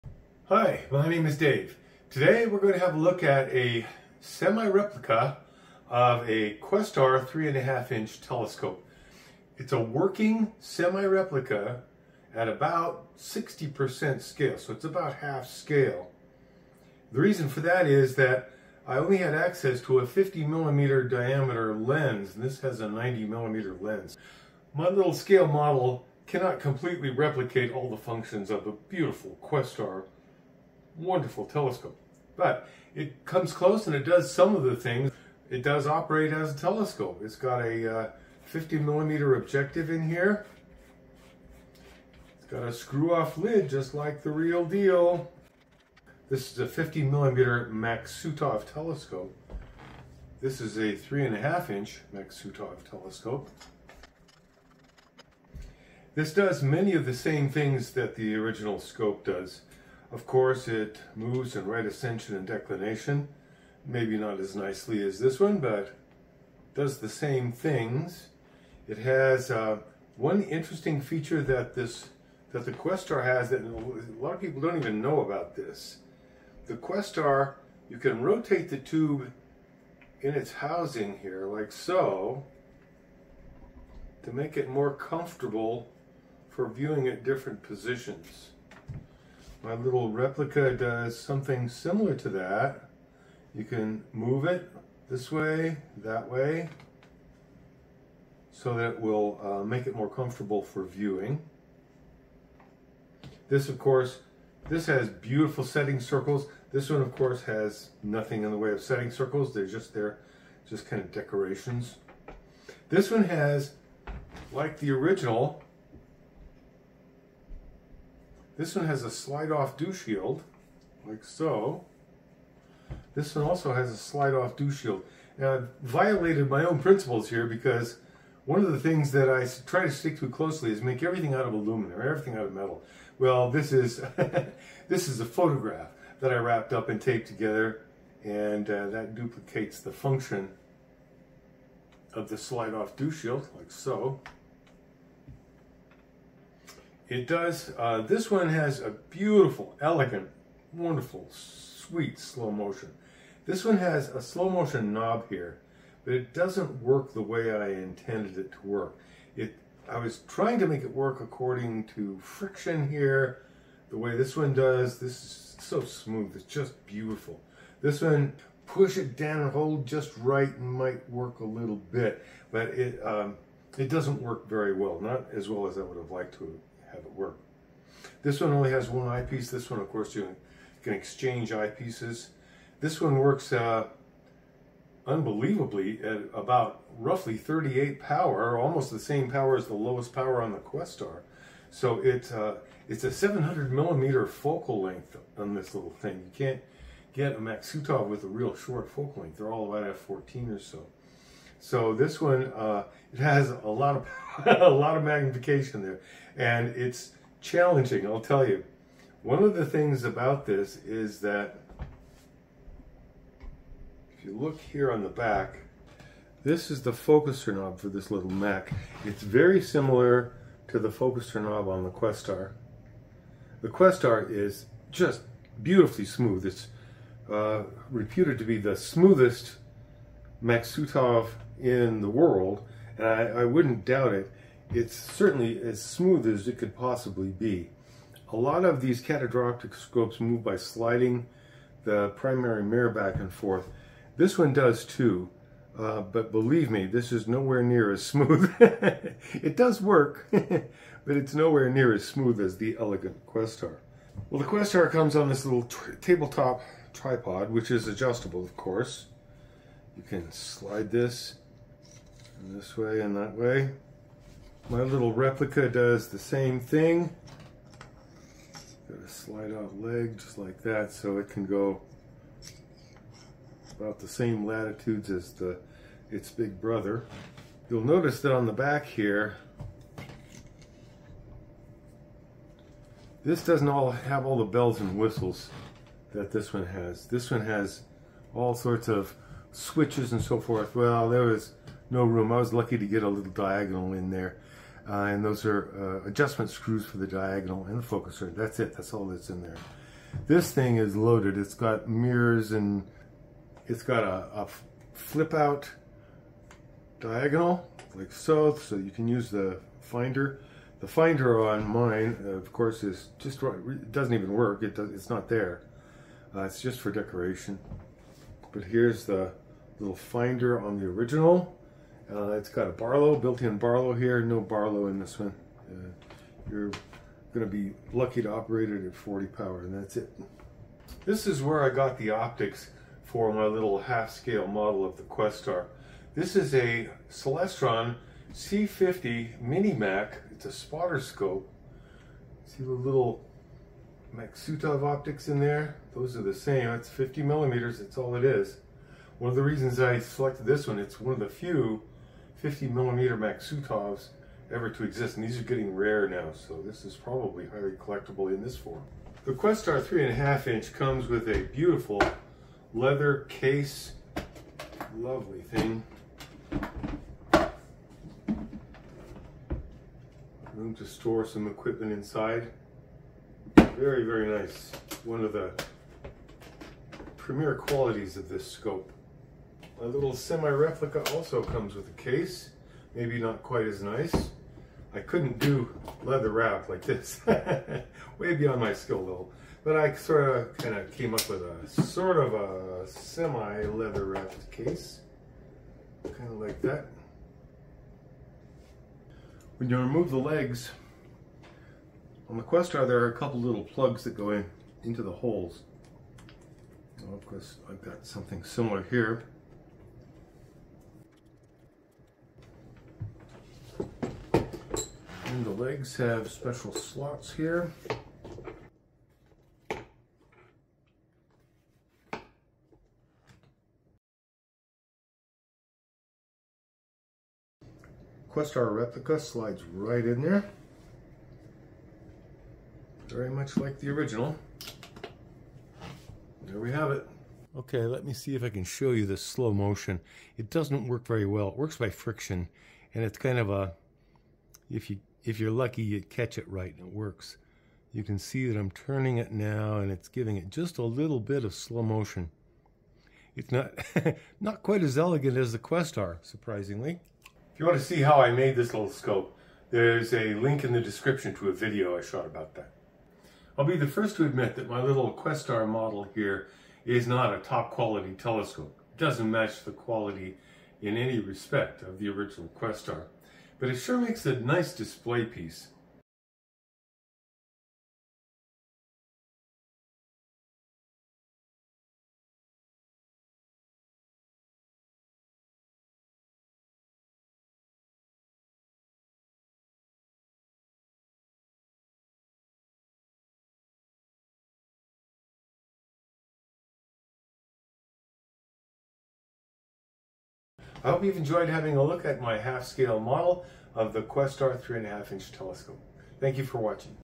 Questar Half Scale Model
This is a model of a Questar 3 ½ inch Classic Telescope. It is done at approximately half the size of the original (60%). The model is a fully functional semi-replica, though it does not have all the features of the original and does not meet the impeccable quality standards of the Questar 3 ½ inch scope. Long considered to be the Rolls Royce of small telescopes, the Questar is renowned among amateur astronomers for good reason. The model is meant as a tribute to the original. It is intended for display and is not intended for daily use. It looks good and fits nicely on a desk or mantle.